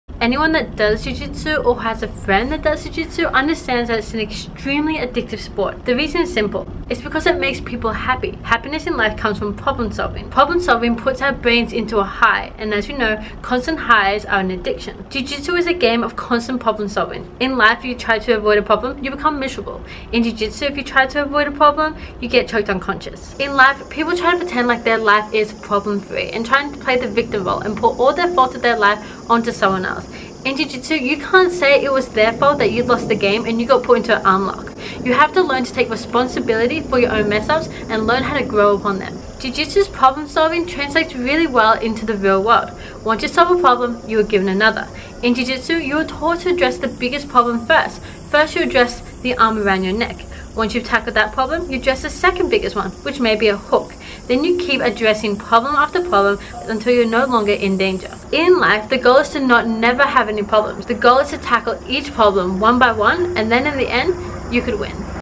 Why you are addicted to Jiu Jitsu
Ever wondered what makes people so addicted to a sport like Jiu jitsu, Its not the sweaty cuddling or the bruises.